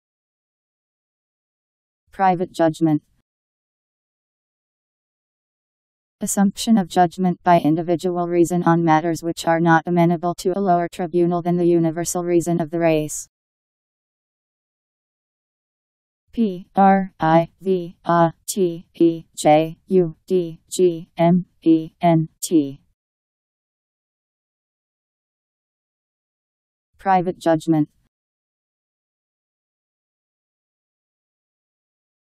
Private judgment Meaning
Video shows what private judgment means. Assumption of judgment by individual reason on matters which are not amenable to a lower tribunal than the universal reason of the race..  Private judgment Meaning. How to pronounce, definition audio dictionary. How to say private judgment. Powered by MaryTTS, Wiktionary